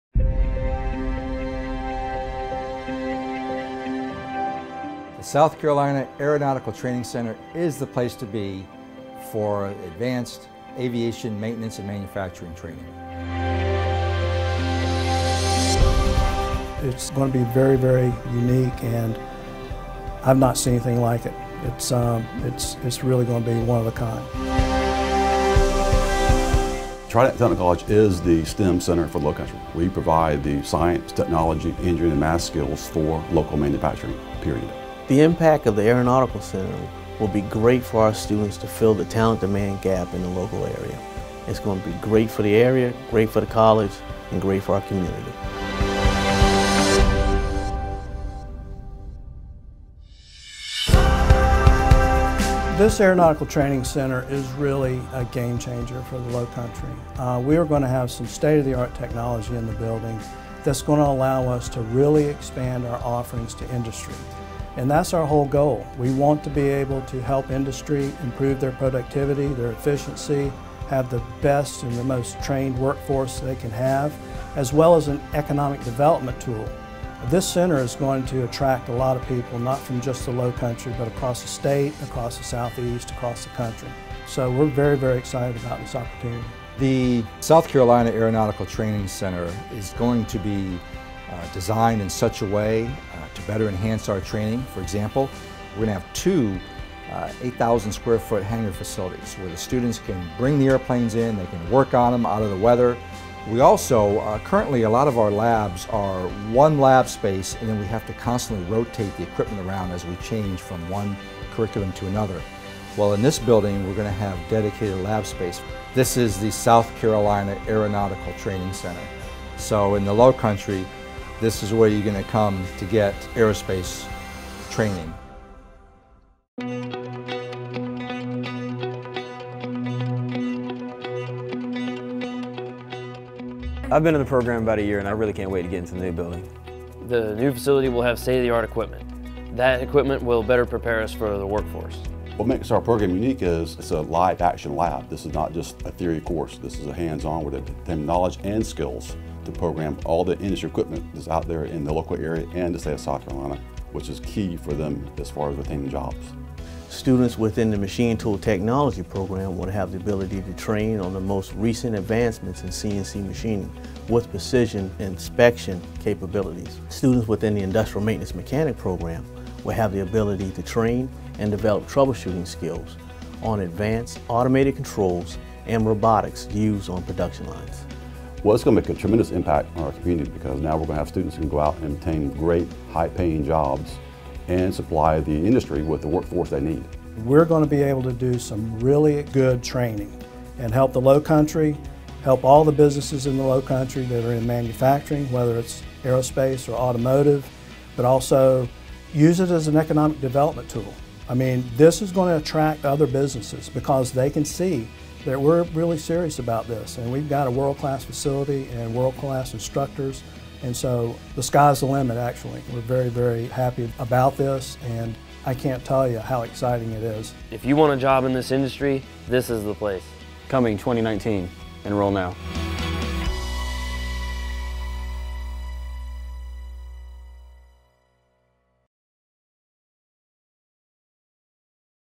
South Carolina Aeronautical Training Center at Trident Technical College
The new South Carolina Aeronautical Training Center at Trident Technical College will provide training for well-paying jobs in expanding advanced manufacturing industries with diverse workforces and excellent job mobility.